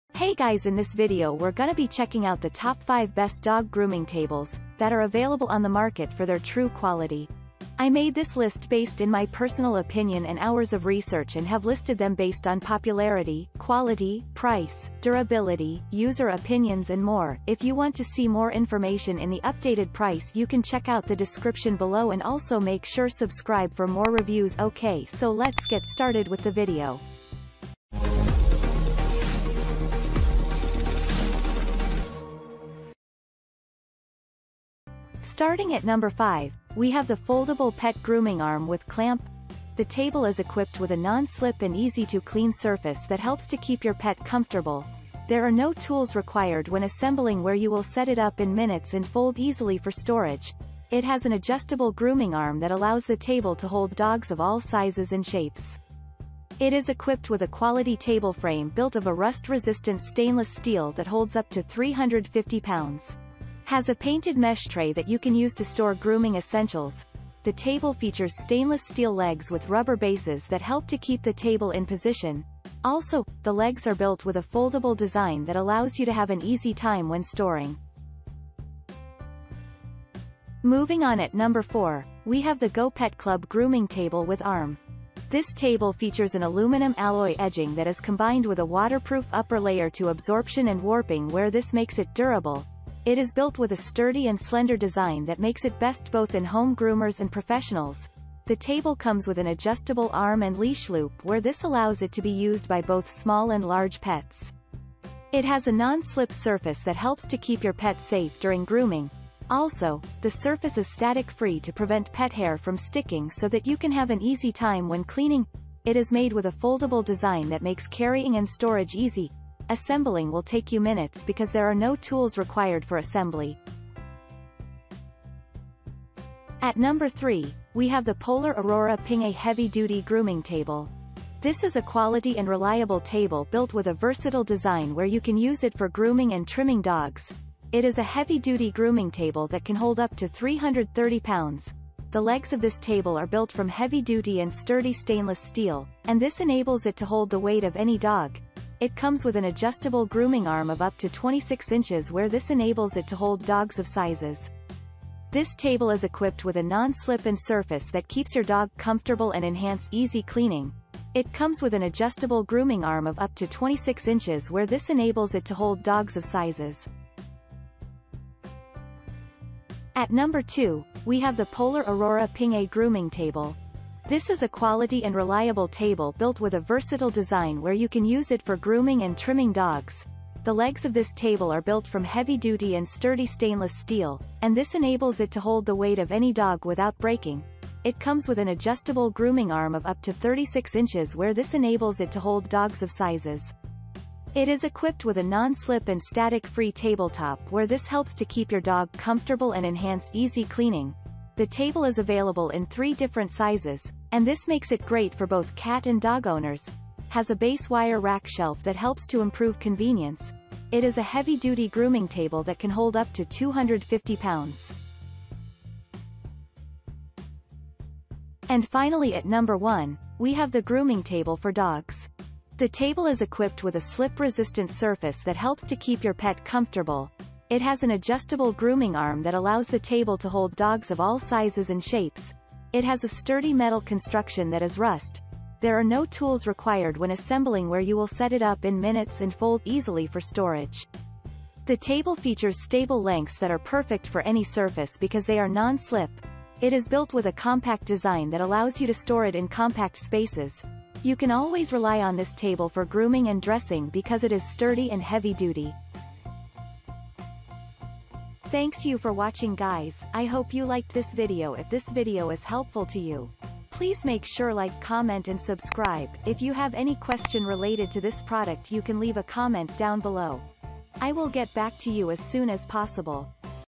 ✅Top 5 Best Dog Grooming Tables Reviews in 2024
▶️ 5. Foldable Pet Grooming Arm with Clamp

▶️ 4. Go Pet Club Grooming Table with Arm

▶️ 3. Polar Aurora Pingkay Heavy-Duty Grooming Table

▶️ 2. Polar Aurora Pingkay Grooming Table

▶️ 1.  Grooming Table for Dogs